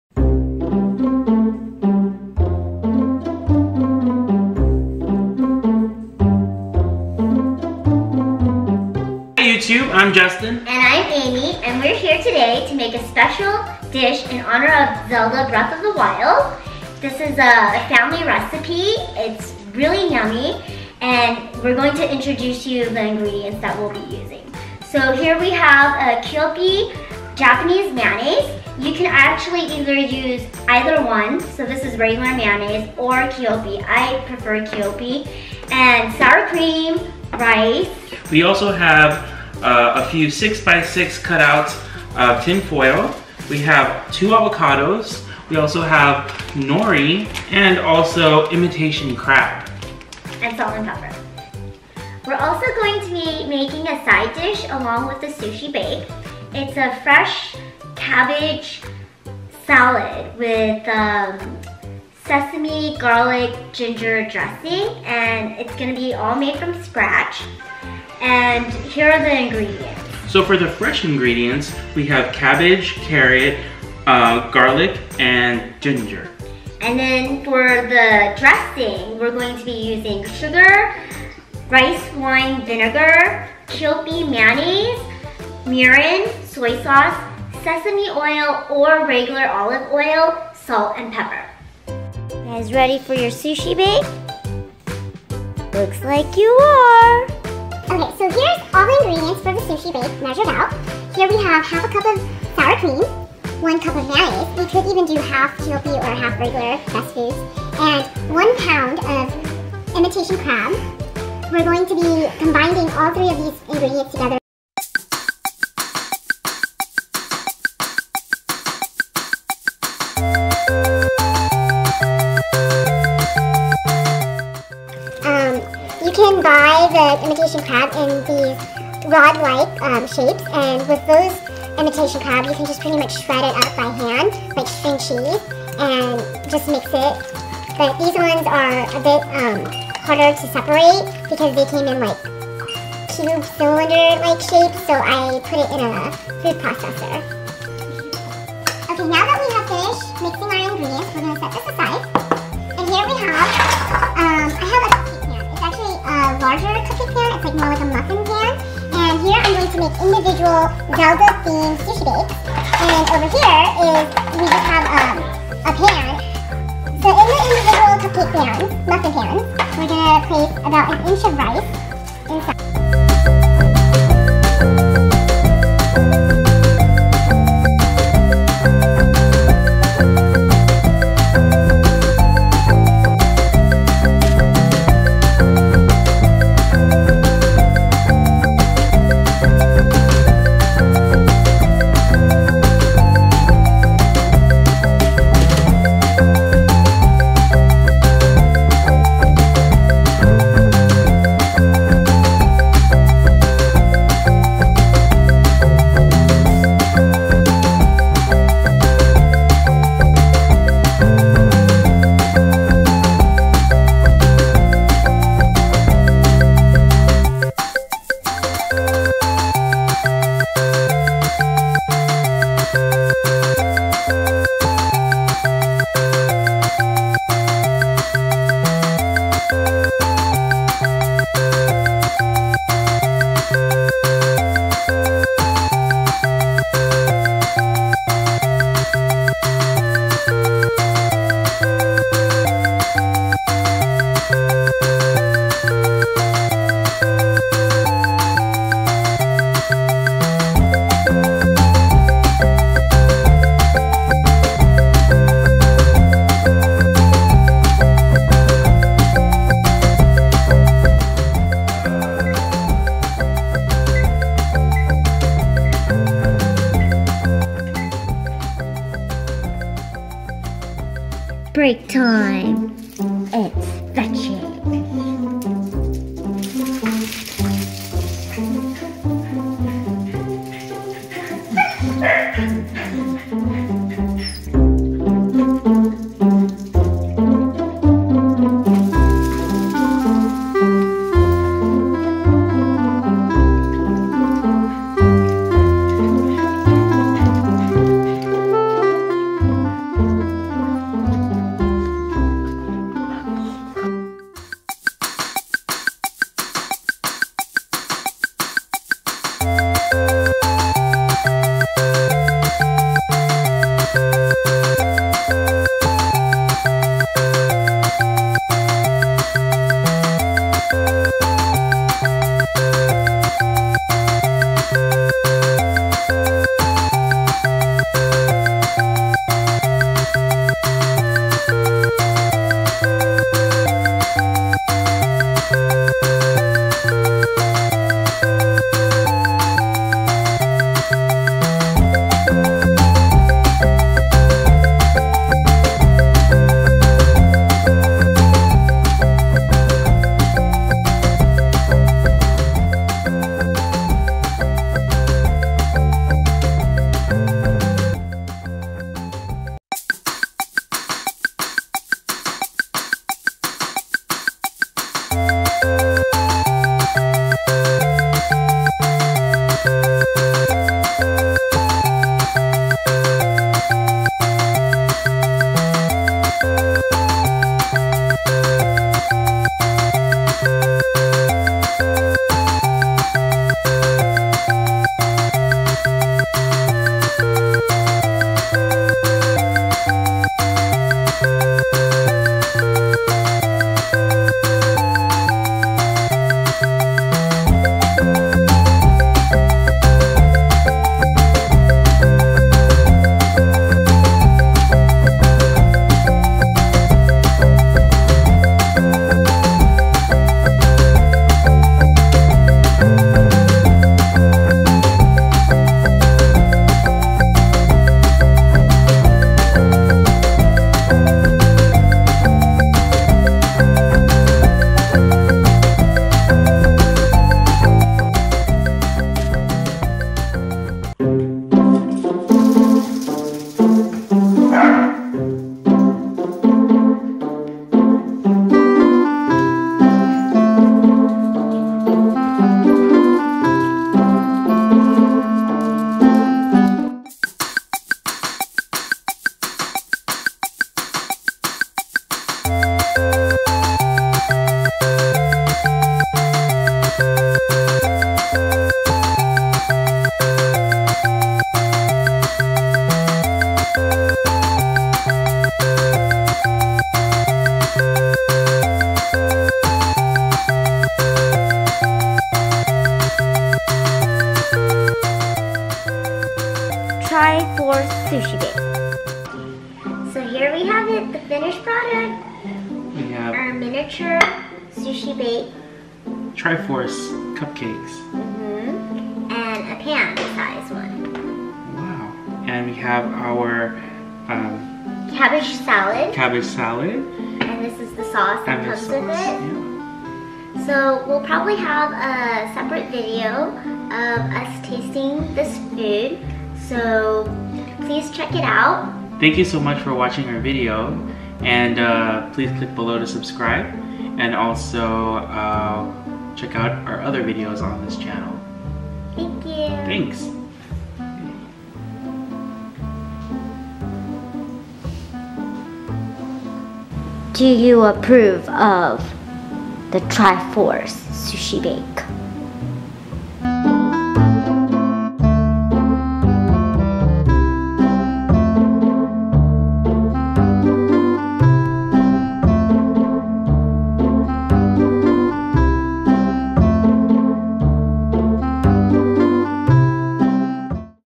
Zelda Sushi Bake: Cooking with the CORGIS!!!
In honor of Zelda: Breath of the Wild, here is a fun simple way to make a sushi dish that can easily impress/feed your family and friends. Here are the ingredients listed below.

Ingredients for the Sushi Bake:
1 to 1.5 lbs Imitation Crab
1/2 lb of salad shrimp (optional)
1 cup of Mayonnaise (Japanese mayo pref.)
1/2 cup Sour Cream
3 cups of steamed rice

Toppings for the Sushi Bake:
Avocado thinly sliced
Fish Roe (fish eggs)
Sushi grade fish thinly sliced (Tuna, salmon, etc.)
Cucumber thinly sliced
Nori Seaweed

Fresh Cabbage Salad:
1 whole Cabbage thinly chopped
1 Carrot shredded

Ginger Garlic Dressing:
1 cup of Mayonnaise (Japanese mayo)
2 Tbsp Sour Cream
1/2 Tbsp Ginger (minced or puree)
1/2 Tbsp Garlic (minced or puree)
1 Tbsp Soy sauce
2 Tbsp Rice wine vinegar
1 Tbsp Mirin
1 Tbsp oil (sesame or olive oil)
2 Tbsp sugar
Pinch of black pepper
1 Tbsp Toasted sesame seeds (optional)